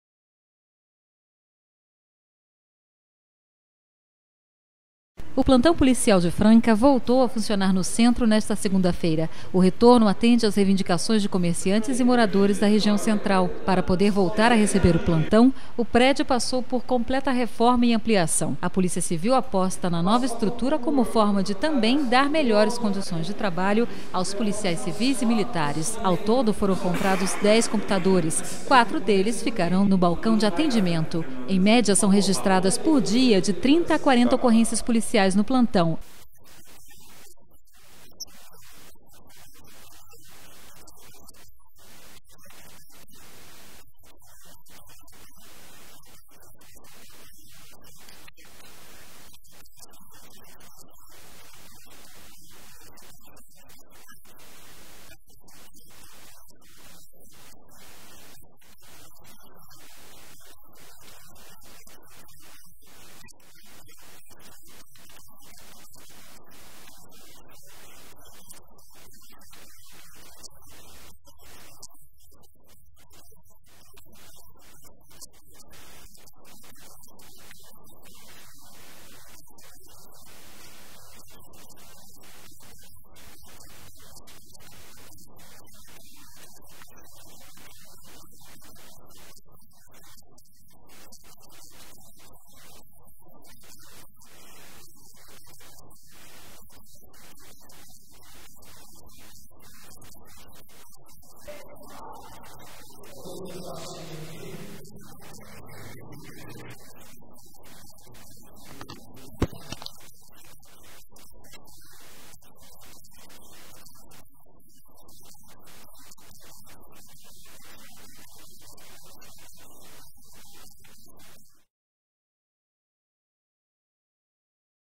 Polícia Civil inaugura Plantão e anuncia mudanças de delegados
O diretor do Deinter-3 Walmir Eduardo Granucci confirmou a mudança de delegados em unidades de Franca e região. O anúncio foi feito durante a cerimônia de inauguração do novo Plantão da Polícia Civil. Marcelo Caleiro, delegado Seccional, foi quem solicitou as medidas. Ele não revelou o nome dos policiais, mas confirmou que as mudanças serão realizadas ainda esta semana como forma de "dinamizar" os trabalhos da Polícia Civil no município. Caleiro também afirmou que em janeiro será inaugurado o 6º Distrito Policial, no Jardim Aeroporto.